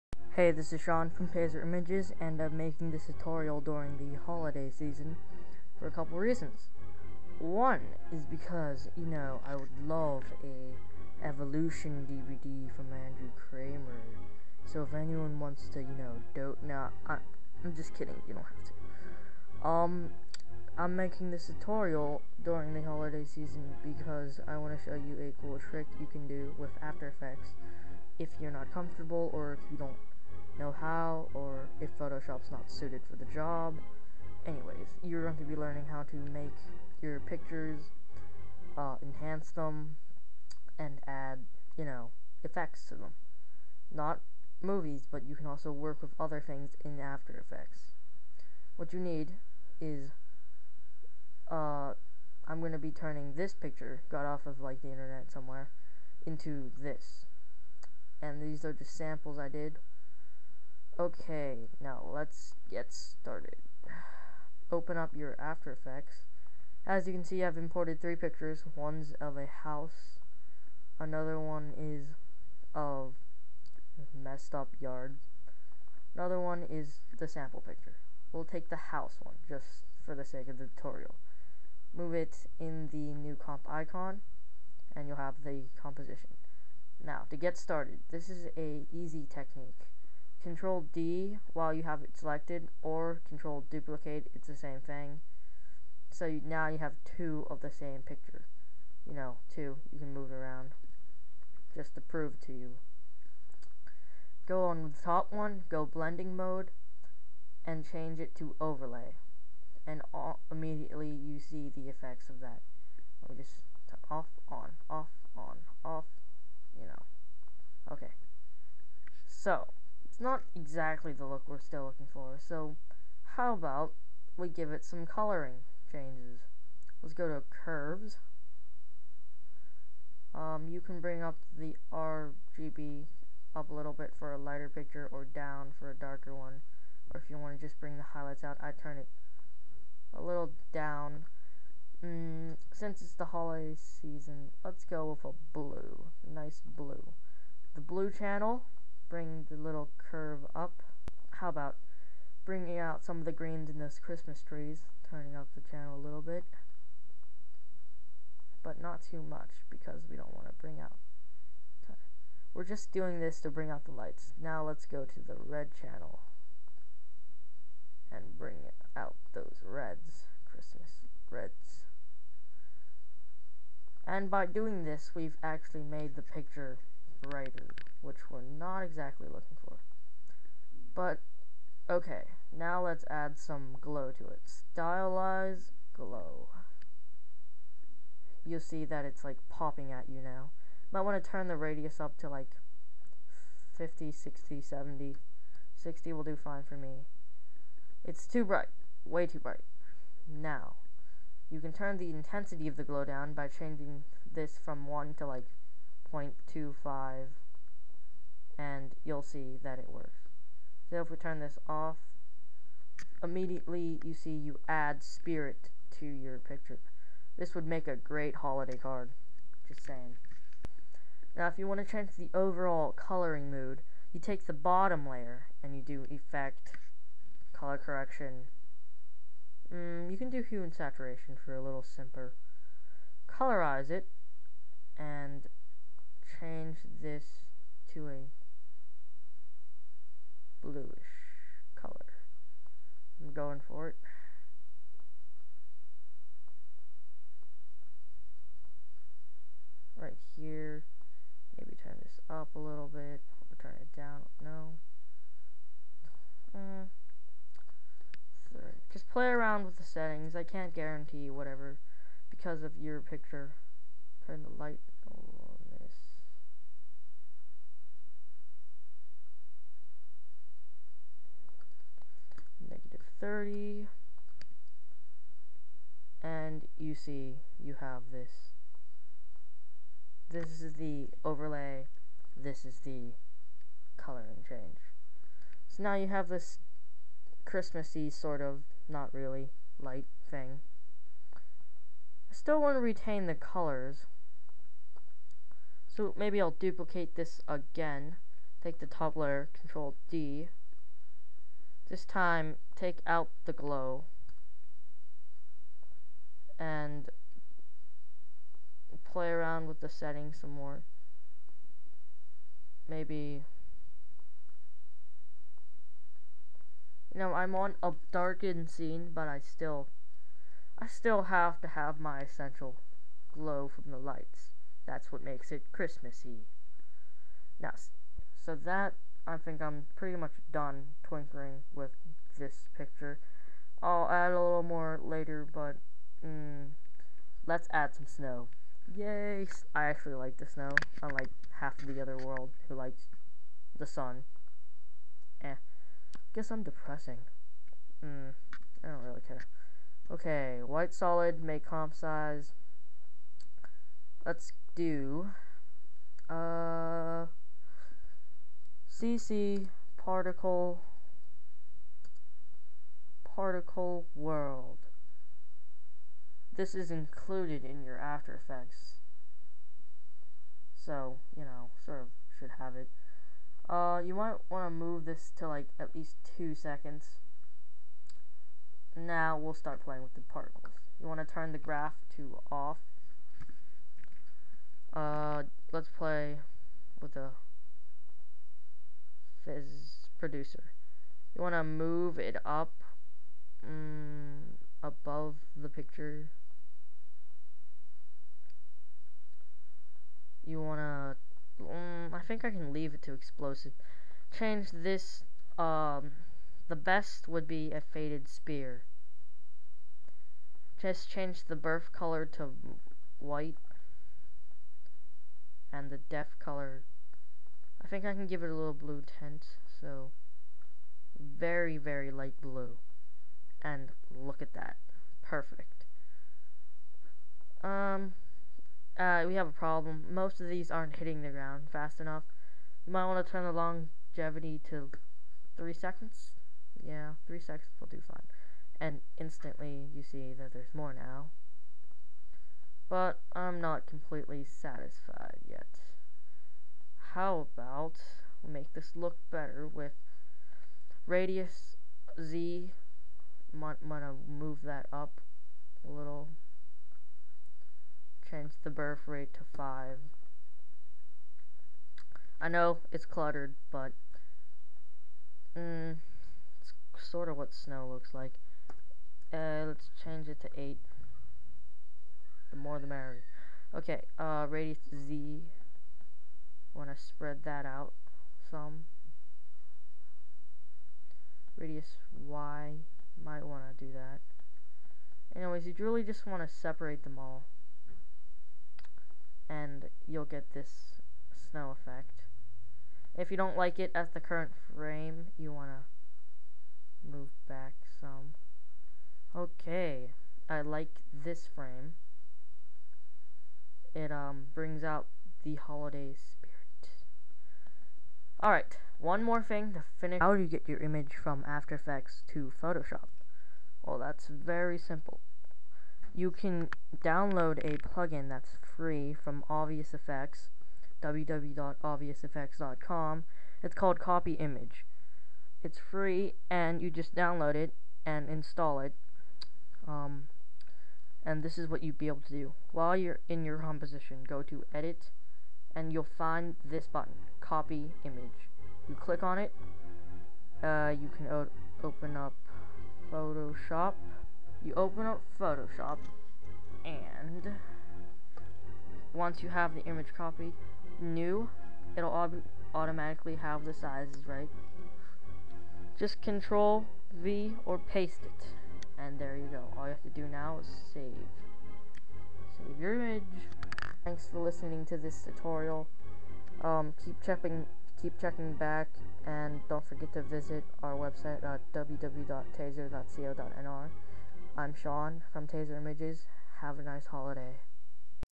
=Holiday Photo Editing=[AfterFX Tutorial]
In this tutorial, we will be learning how to edit your holiday photos and enhance them on After Effects, thats right, Adobe After Effects. Not Photoshop. OMG!

Enjoy and Happy Holidays!